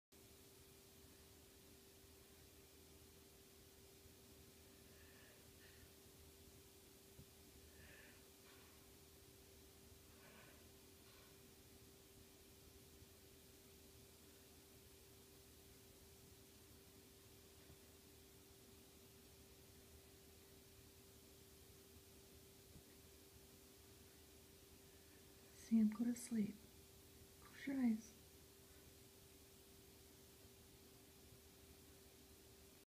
My dog gets messed up from muscle relaxers
Poor little guy had a back spasm. Looks like his meds just kicked in!